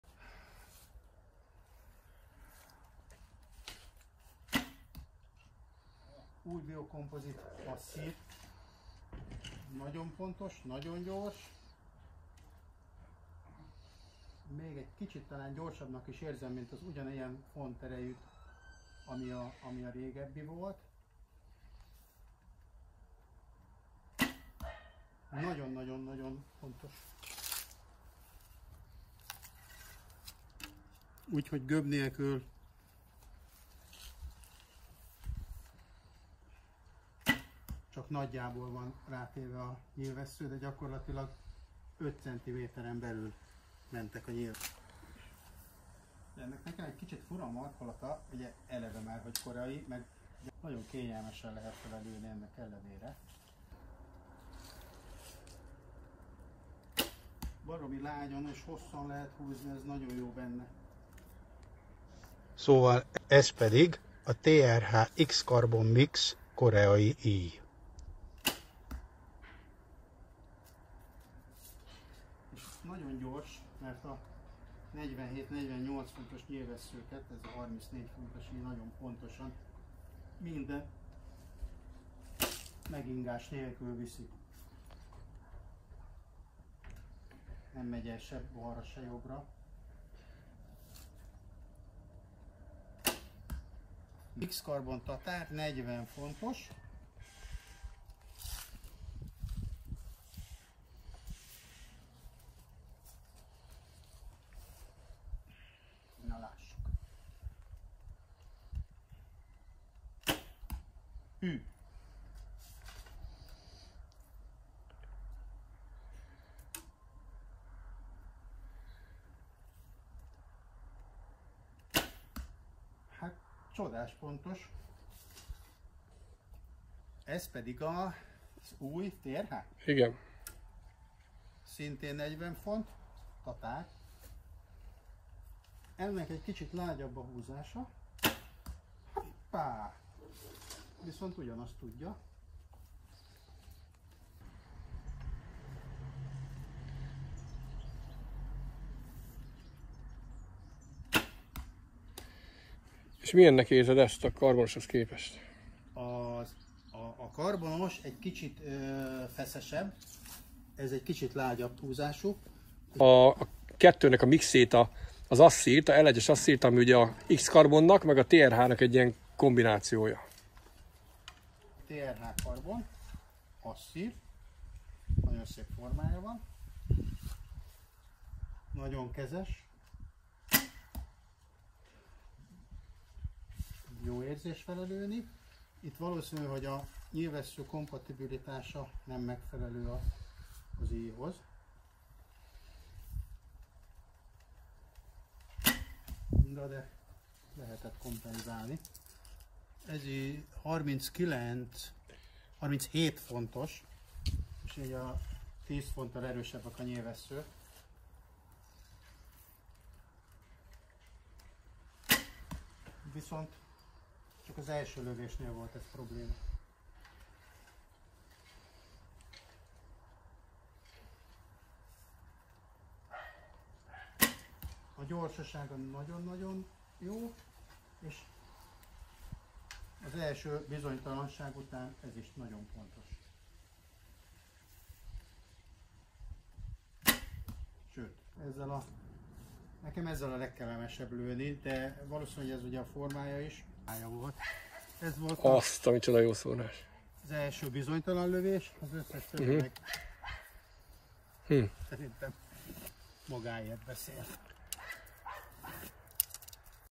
Archer Zsolt Kovács, multiple World ,European and National Champion, tests the new Grózer bows.
Grózer Csaba íjkészítő videói
Csaba Grózer Bowyer master videos